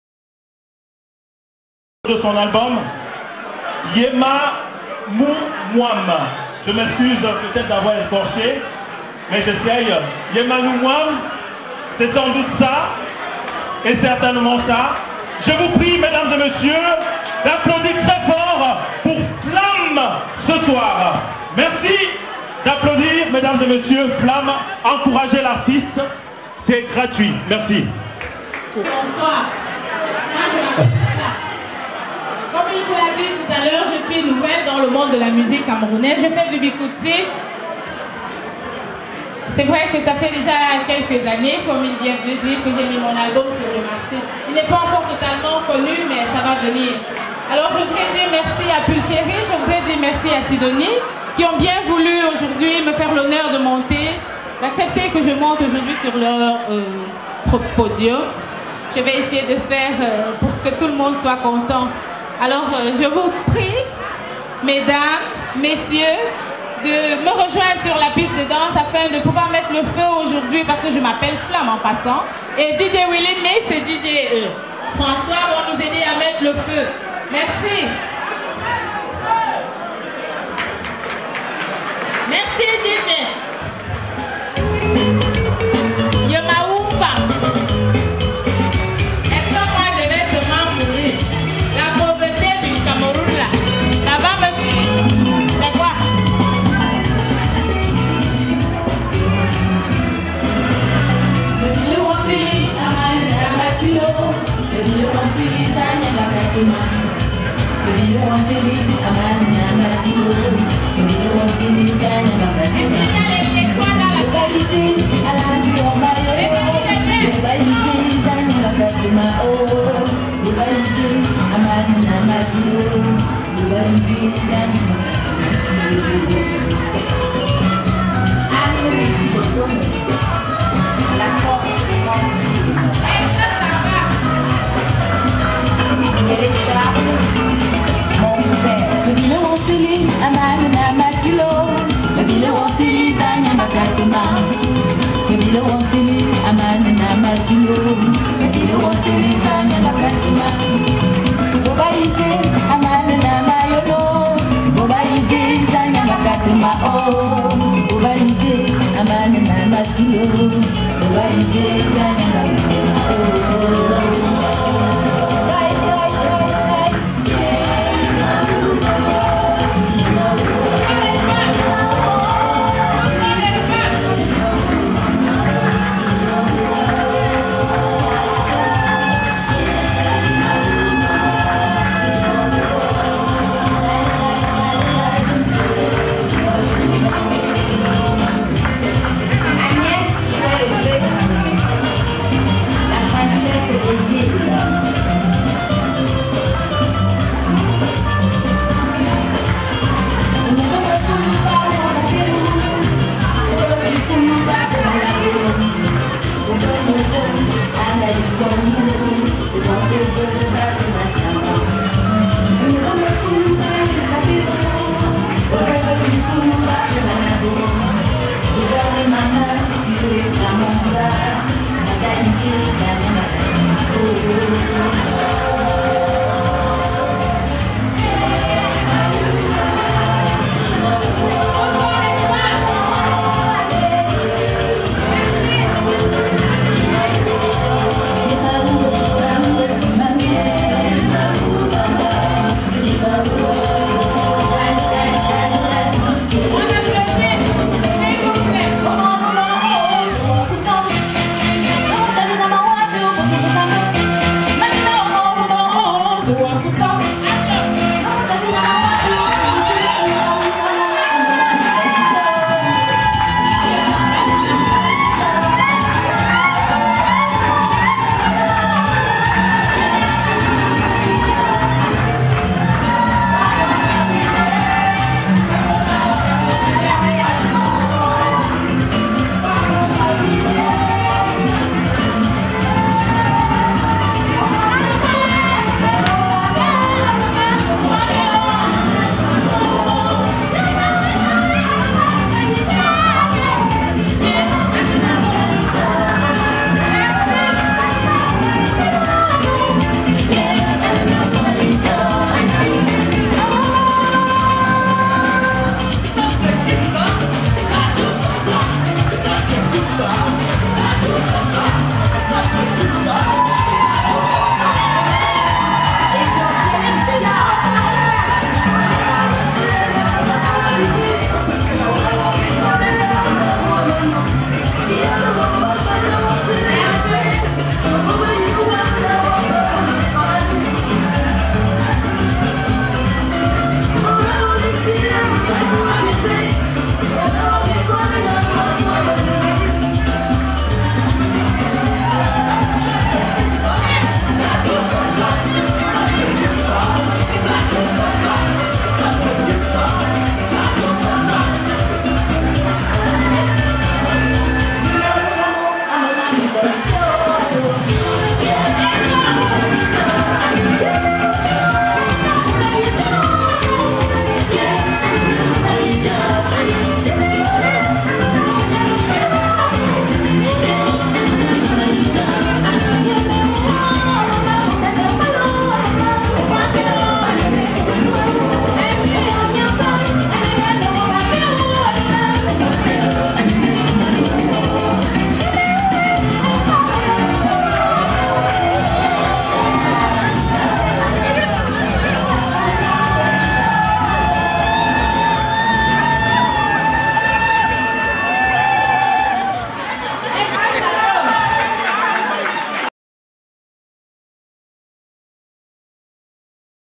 "La Douce Fla-M en concert à Bruxelles" par Ledoux Paradis
La Douce Fla-M en Concert à Bruxelles. Concert Organisé par Sidonie & Pulcherie
Image - Montage - Réalisation: Ledoux paradis pour le Studio Télé SPI"
Auteur compositeur: Ledoux paradis
pour l'ASBL "Télé SPI" (Solidarité Paradisiaque Internationale)
Dampremy Charleroi Belgique